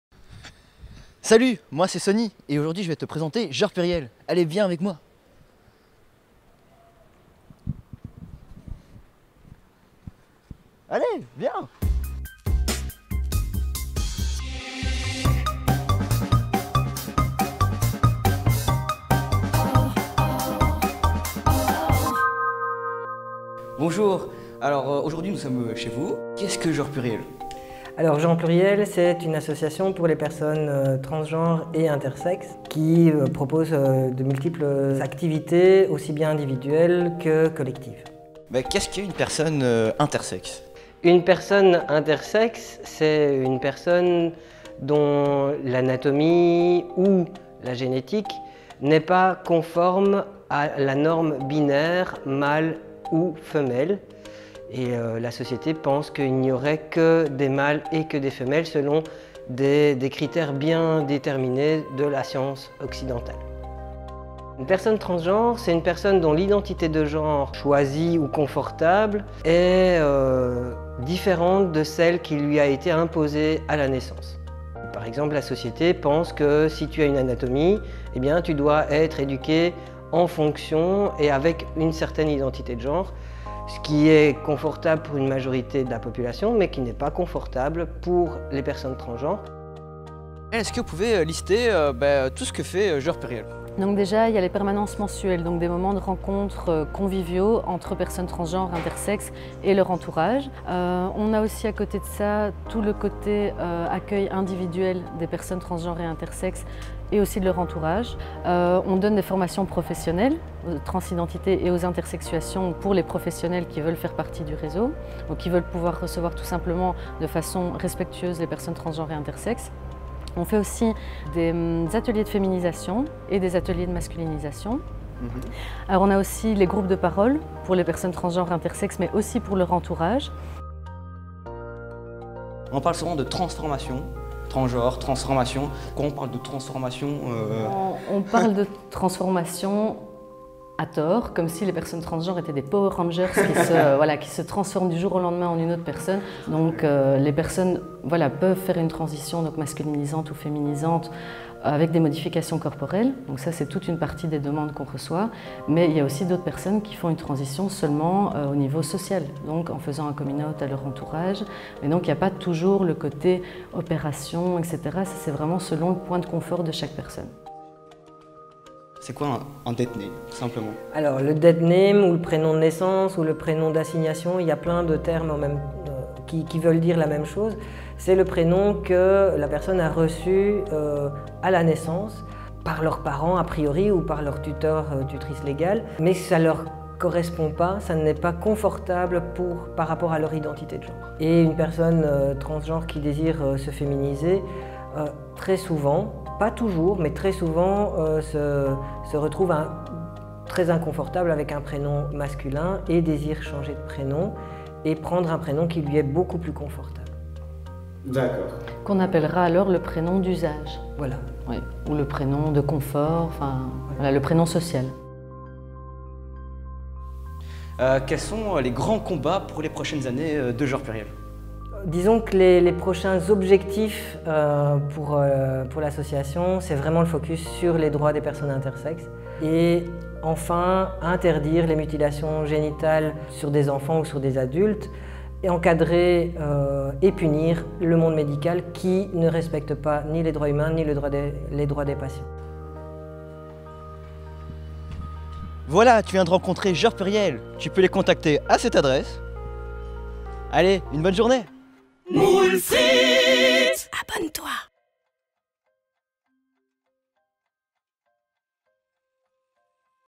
Sex’Pertise #4 : Genres Pluriels
Genres Pluriels, c’est l’association qui lutte pour la visibilité des personnes aux genres fluides, trans et intersexes.